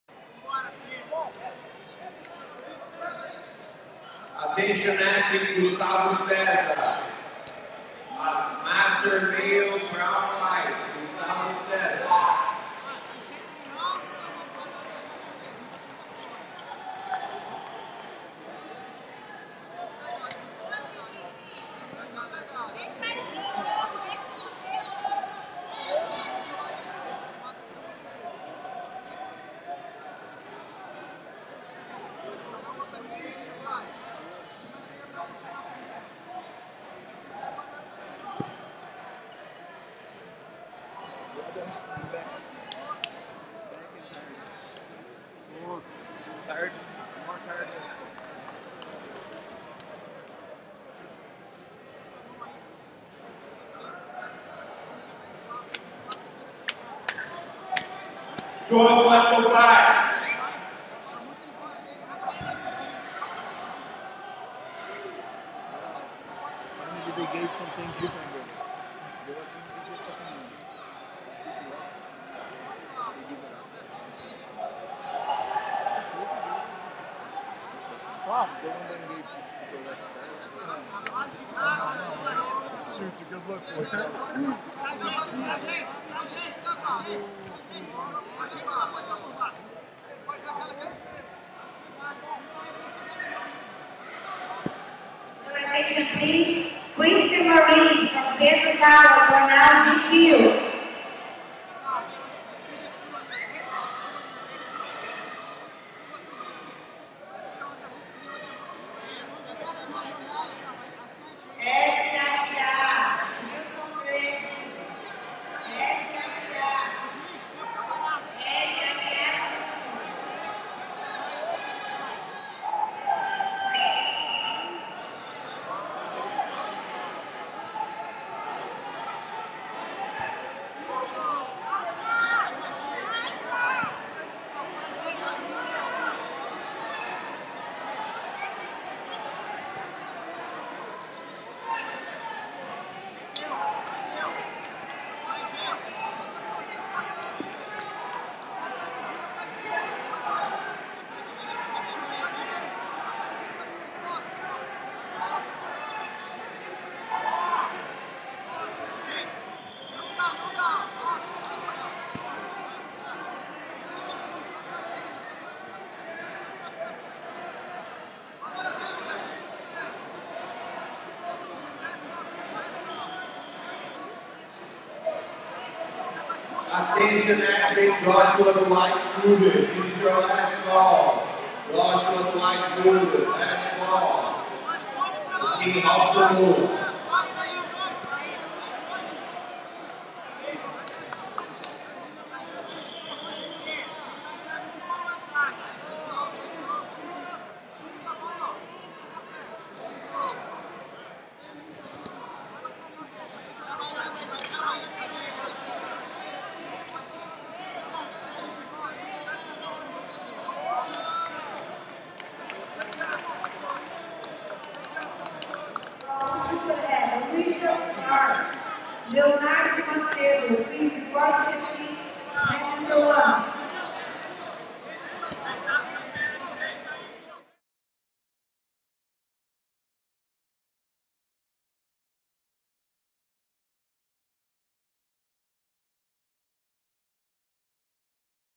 Gilson Nunes v Felipe Liborio / World Master 2020
MASTER 1 / BLACK-BELT / MALE / LIGHT - SEMIFINAL